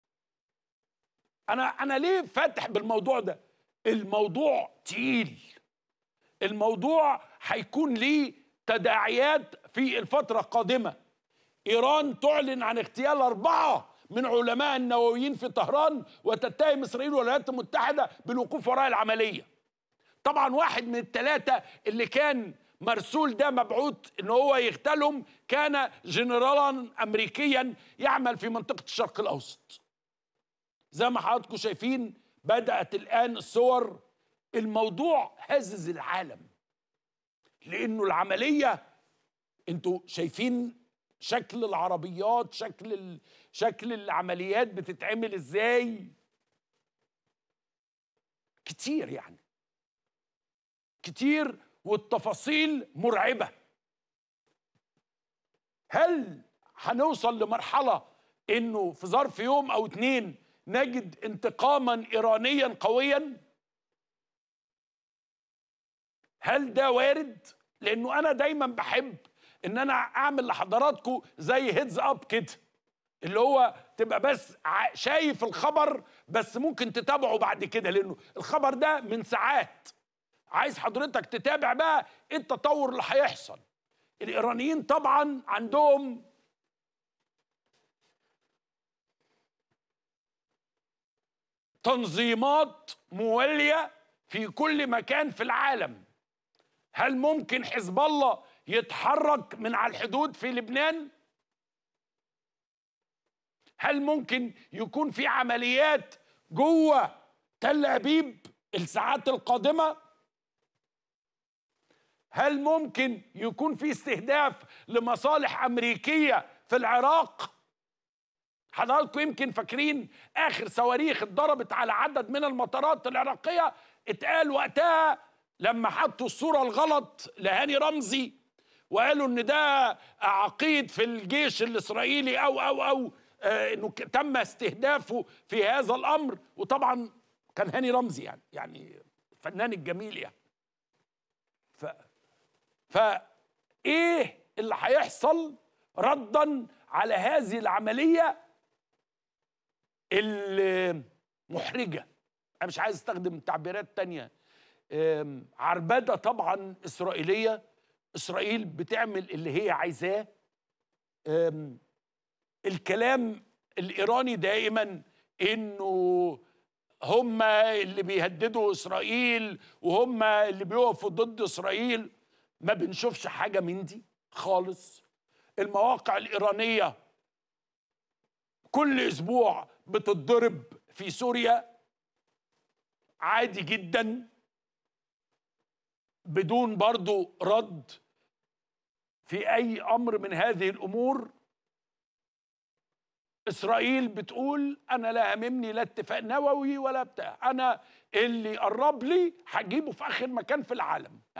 عمرو أديب: إسرائيل اغتالت ٤ علماء نوويين غير عقيد في الحرس الثوري .. إيران ولا حزب الله هيردوا إزاي؟
عمرو أديب: إسرائيل اغتالت ٤ علماء نوويين إيرانيين غير عقيد في الحرس الثوري .. إيران ولا حزب الله هيردوا إزاي؟